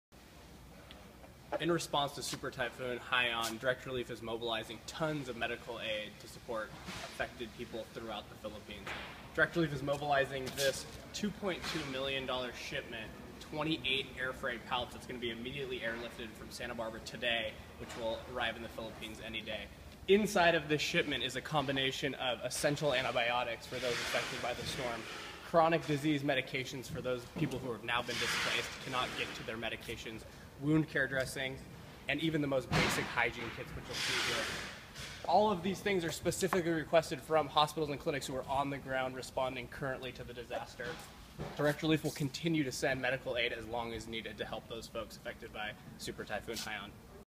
Direct Relief's Typhoon Haiyan Response
Direct Relief's Director of Emergency Response, Brett Williams, discusses the relief efforts so far.